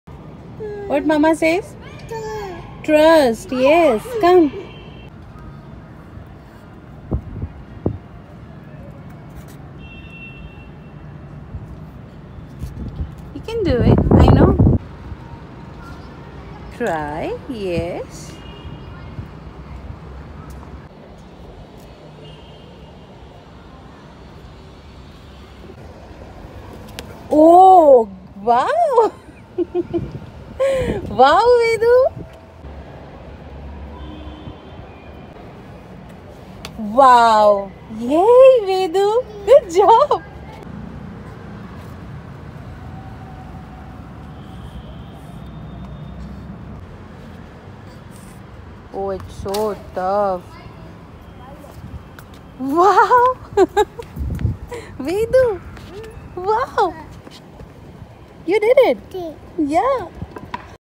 Brave Girl with Down-Syndrome Conquers Stairs for the First Time || ViralHog
Occurred on February 17, 2020 / Ghaziabad, India

"Trust Yourself. A 4 year old girl with Down Syndrome and low muscle tone was trying to walk down the stairs with support for the first time. She's putting a brave face and trying to step down, which is the toughest thing for her right now as she needs good quad muscles strength, knee control and weight bearing at the same time to do that. That's why her mom is telling her to TRUST herself. What matters is that she didn't give up and conquered her fear."